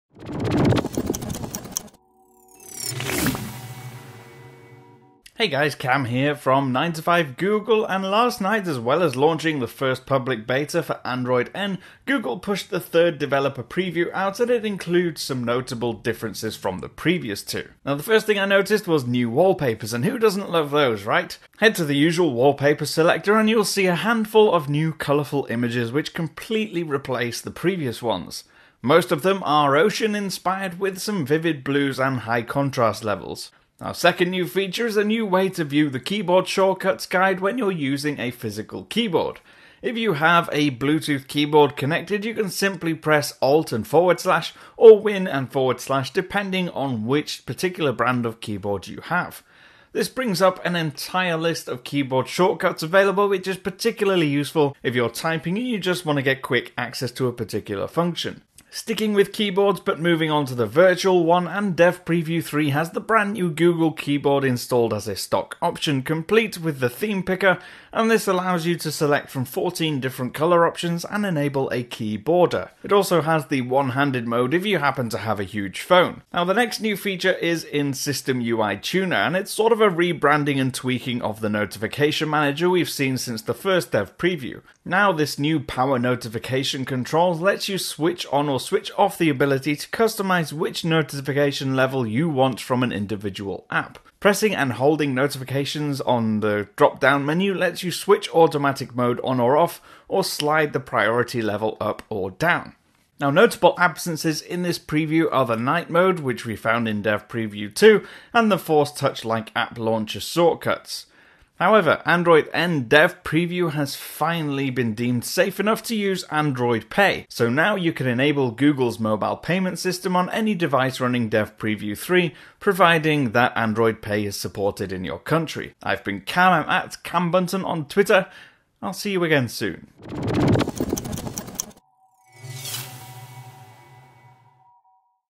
Android N Dev Preview 3 overview - What's new?
EDIT - There is night mode, it now has its own icon in quick settings. Drop down the quick settings from the top, hit 'edit' then drag and drop the 'Night Mode' icon to your selection of tiles. It doesn't offer a 'dark mode' however.

Last night, as well as launching the first public beta for Android N, Google pushed the third developer preview out and it includes some notable differences from the previous two.